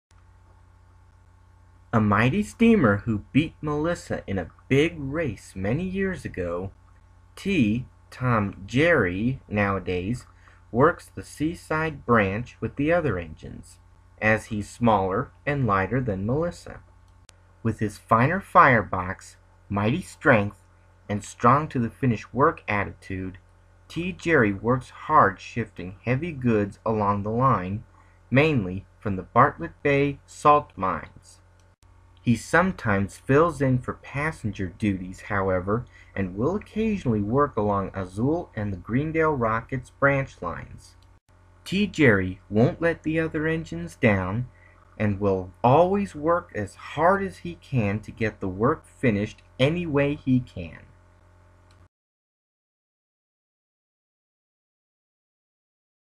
23.T Jerry's BIO read by Duel Express for The Railways of Crotoonia
Crotoonian Property owned by respected owners.

T. Jerry the Seaside Tender Engine:
A mighty steamer who beat Melissa in a big race many years ago, T. (Tom) Jerry nowadays works the Seaside Branch with the other engines, as he's smaller and lighter than Melissa. With his finer firebox, mighty strength and "strong-to-the-finish" work attitude, T. Jerry works hard shifting heavy goods along the line, mainly from the Bartlett Bay Salt Mines. He sometimes fills in for passenger duties however, and will occasionally work along Azul and the Greendale Rocket's Branch Lines. T. Jerry won't let the other engines down, and will always work as hard as he can to get the work finished anyway he can.